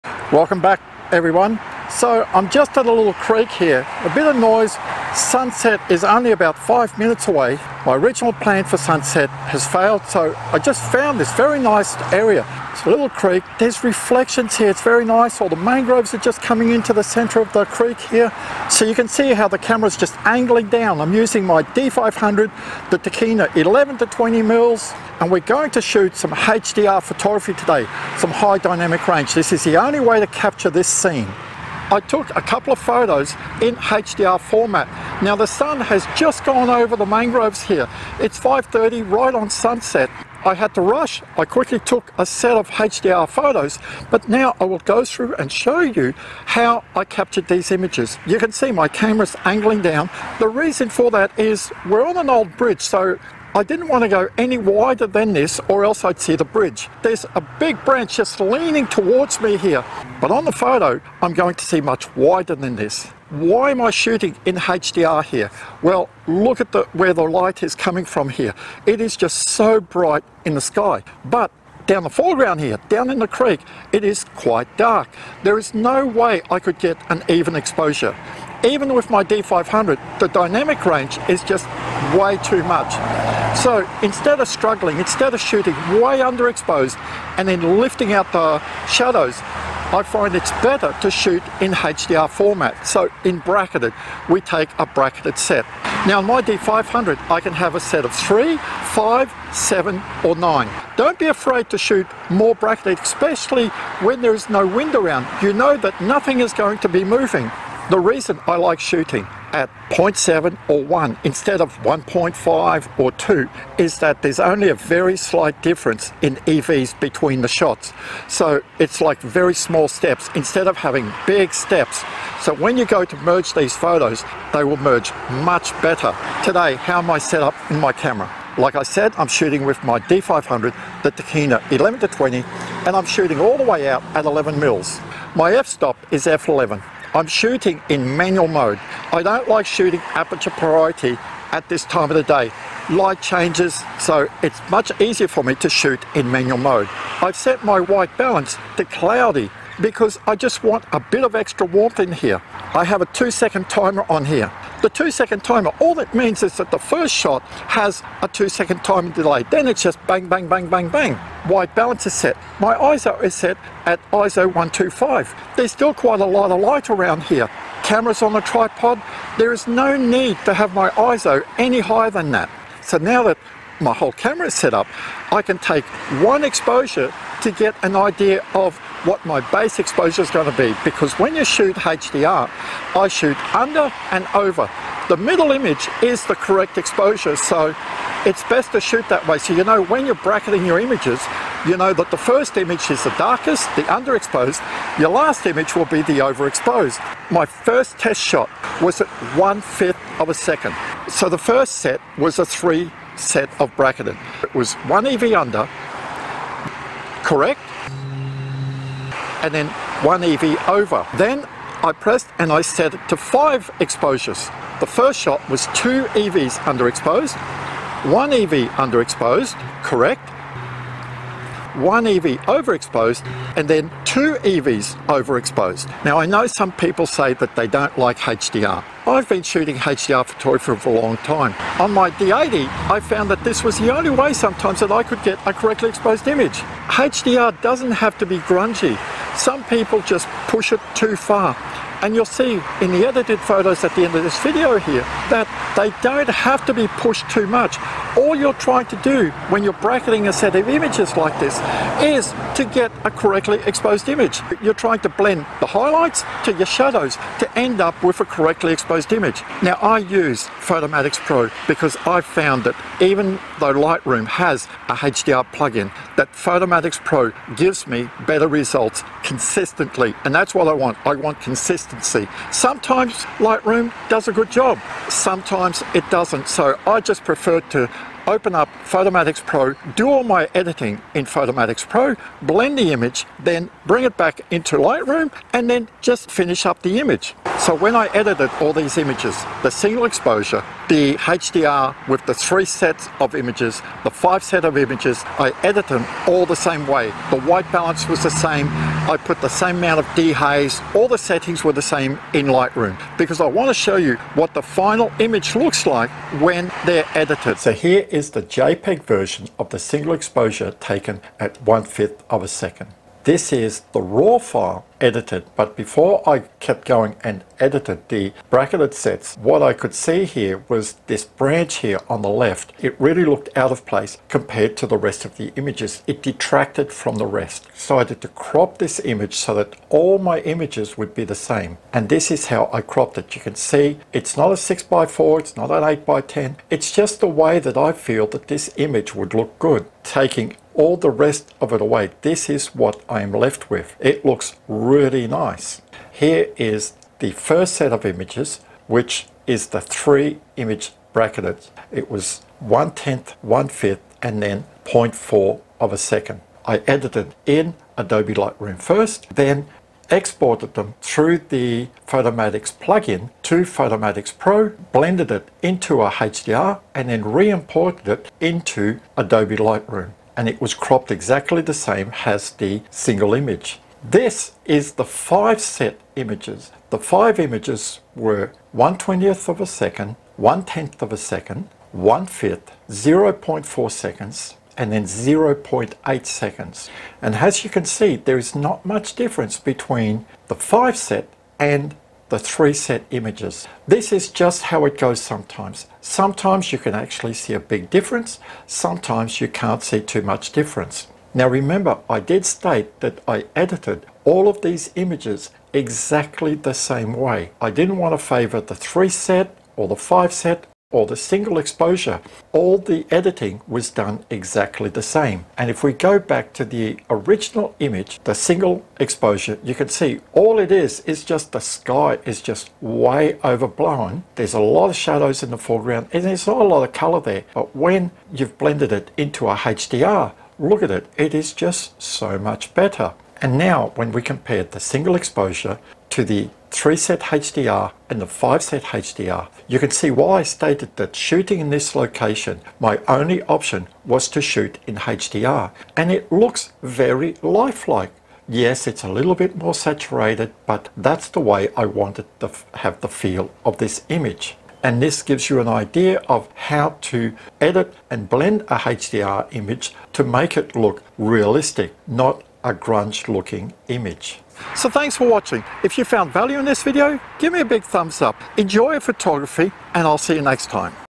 HDR Photography Is It Needed In 2021
HDR Photography is a very divisive topic and some people frown on it, saying that the resulting image looks Fake or Over-processed. This is because when you have to tone down your editing before you render the images into a composite one.

This HDR Photography Tutorial shows how I shot a set of image and edited them in Lightroom and then rendered the images in Photomatix Pro to give me a correctly exposed image on this creek.

I’ve shot HDR “High Dynamic Range” images for over 12 years and in the early days of DSLR’s the camera’s dynamic range was very limited and this was often the only way to get a great sunset or sunrise image.

Even today using my D500, I find there are times that the only way to get the image that I want is to shoot a bracketed set of images and the render them into a HDR image.